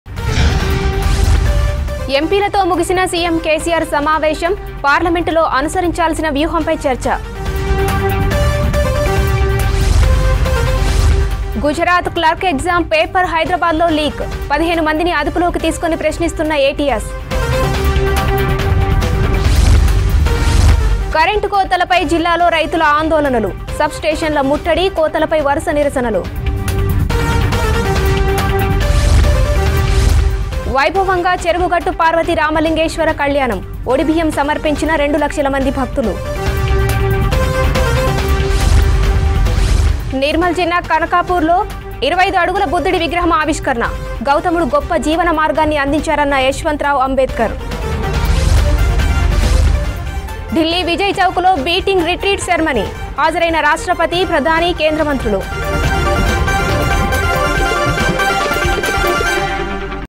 Headlines : CM KCR Meet Ends | Gujarat Clerk Exam Paper Leak| Beating Retreat 2023 | V6 News
Headlines : CM KCR Meet Ends  Gujarat Clerk Exam Paper Leak  Farmers On Power Cuts  Beating Retreat 2023 | V6 News